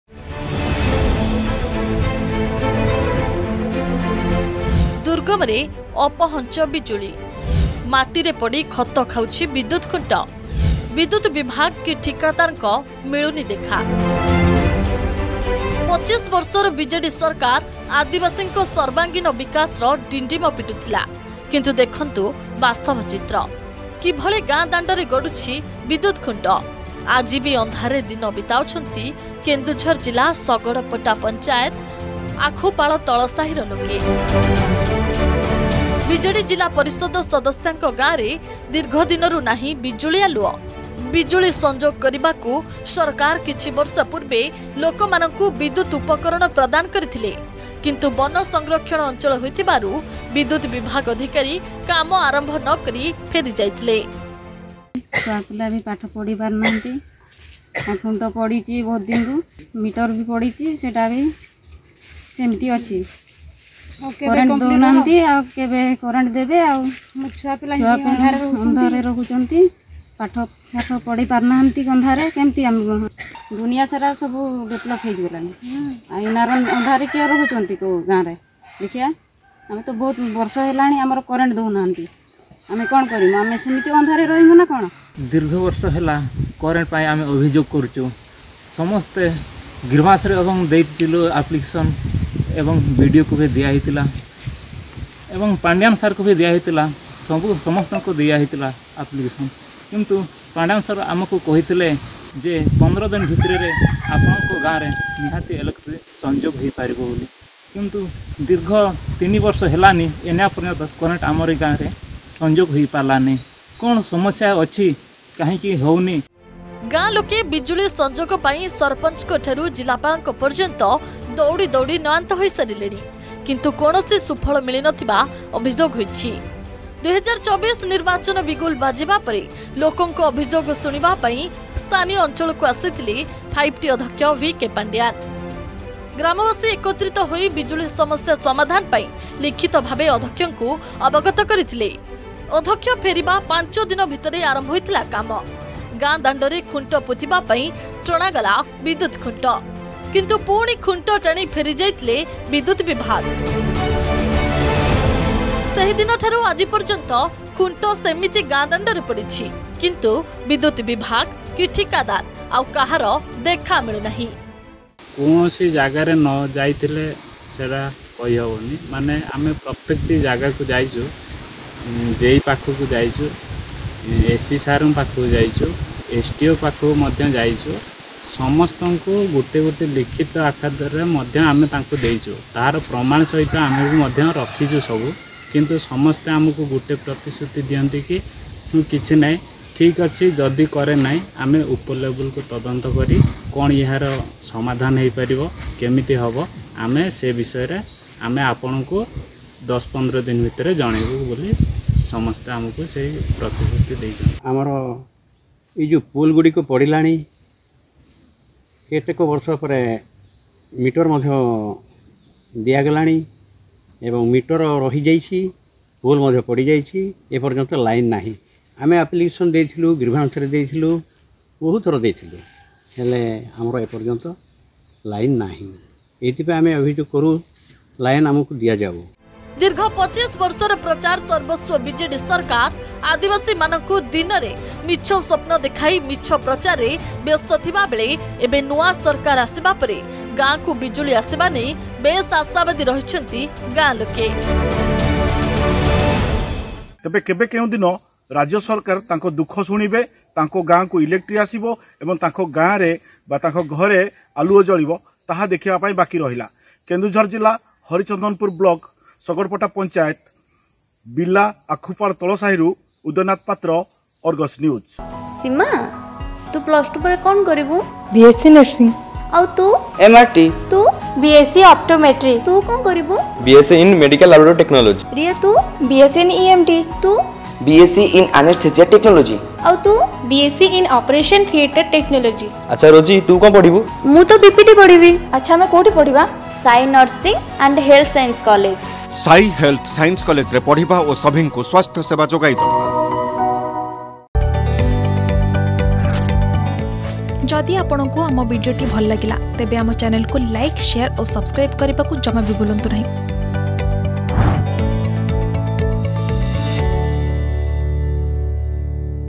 ଦୁର୍ଗମରେ ଅପହଞ୍ଚ ବିଜୁଳି || Keonjhar Harichandanpur Public Issue
25 ବର୍ଷର ବିଜେଡି ସରକାର ଆଦିବାସୀଙ୍କ ସର୍ବାଙ୍ଗୀନ ବିକାଶର ଡିଣ୍ଡିମ ପିଟୁଥିଲା । କିନ୍ତୁ ବାସ୍ତବ ଚିତ୍ର ପୁରା ଓଲଟା। ଗାଁ ଦାଣ୍ଡରେ ଗଡ଼ୁଛି ବିଦ୍ୟୁତ ଖୁଣ୍ଟ । ଆଜି ବି ଅନ୍ଧାରରେ ଦିନ ବିତାଉଛନ୍ତି କେନ୍ଦୁଝର ଜିଲ୍ଲା ଶଗଡପଟା ପଞ୍ଚାୟତ ଆଖୁପାଳ ତଳସାହିର ଲୋକେ ।